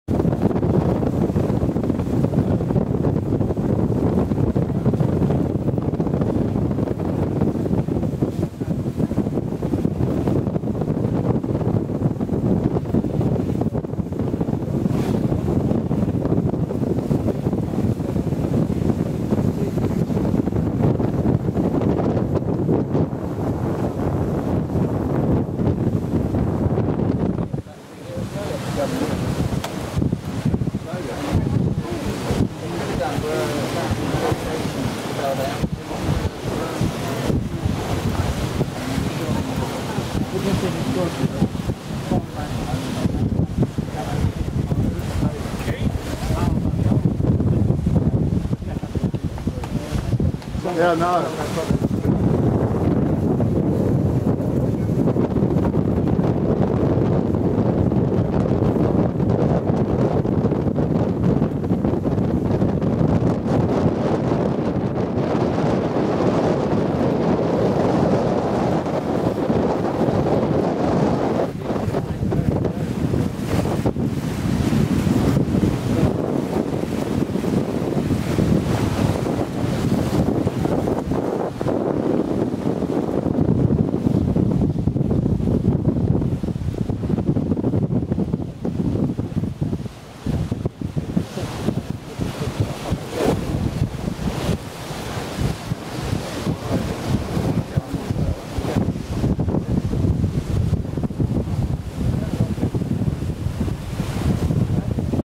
BRIXHAM In Devon. VIGILANCE OF BRIXHAM
Visitors sail Aboard the vigilance. and the crew beatings will continue..spot the T Shirt...